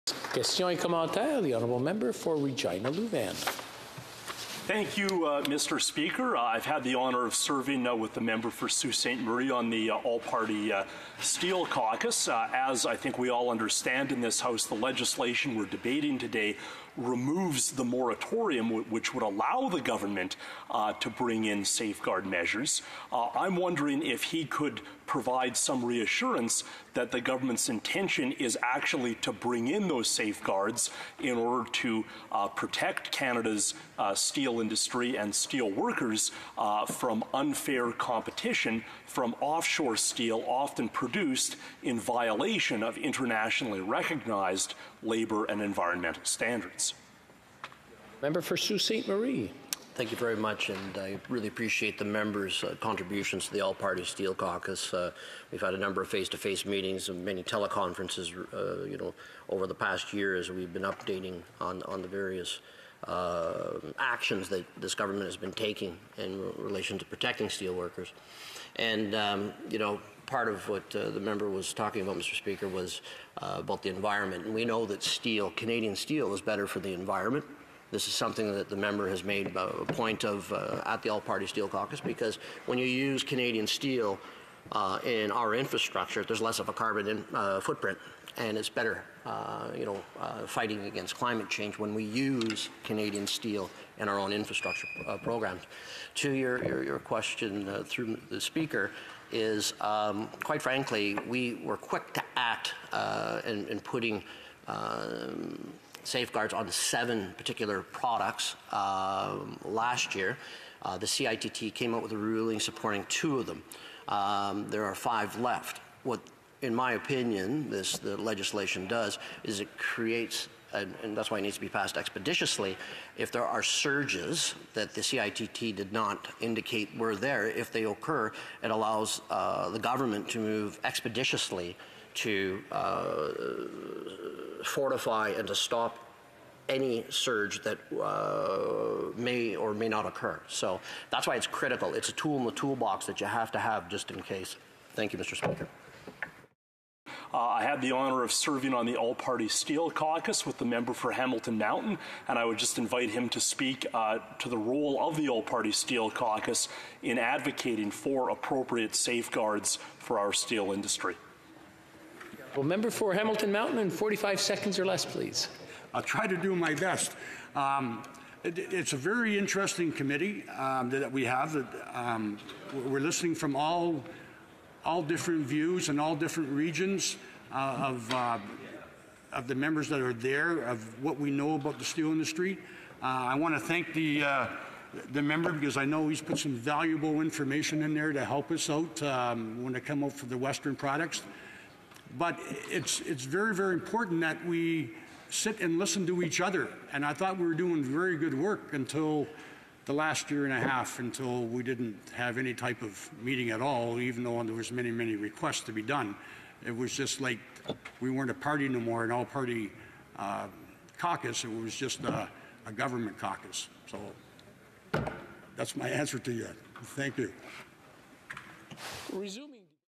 MP Weir on Steel Safeguards (June 6, 2019)
Regina–Lewvan MP Erin Weir discusses measures to safeguard Canadian mills from unfairly-traded offshore steel.